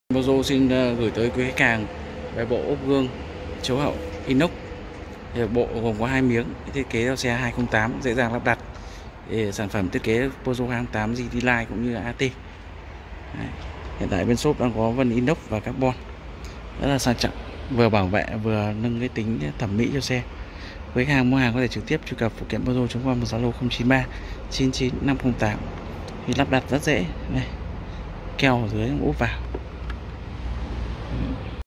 Ốp gương chiếu hậu xe Peugeot 2008 innox
Ốp gương chiếu hậu carbon xe Peugeot 2008
Thiết kế riêng vừa vặn chuẩn xác cho xe Peugeot 2008
Hoàn thiện cao cấp, tỉ mỉ, gương sau khi ốp hoạt động hoàn hảo.
Lắp đặt dễ dàng và chắc chắn, độ bền cao
Bề mặt carbon bóng bẩy, chống trầy xước, bền bỉ dưới mọi điều kiện.